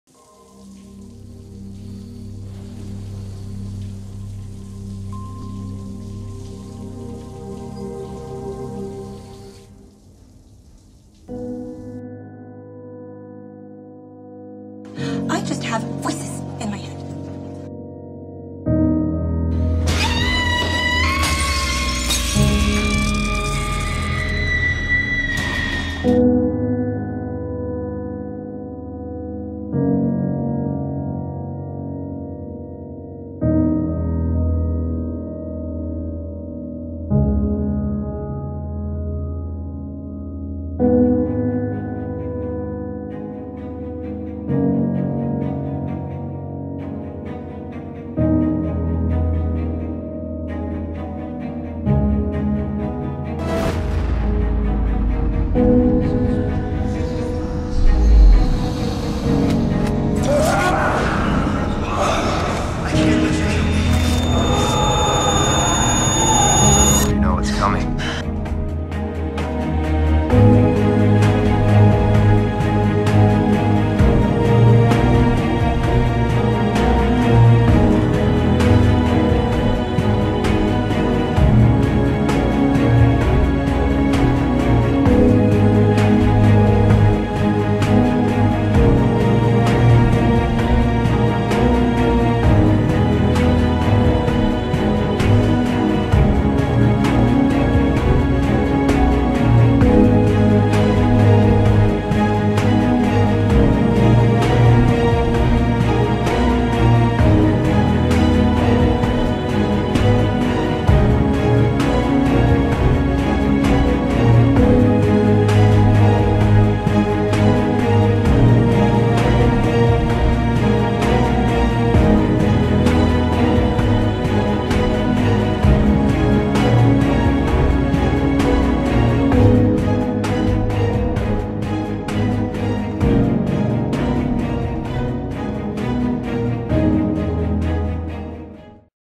Voices | Enoch O'Connor Trailer
I made a trailer for a fanfiction on Wattpad. Link below ;)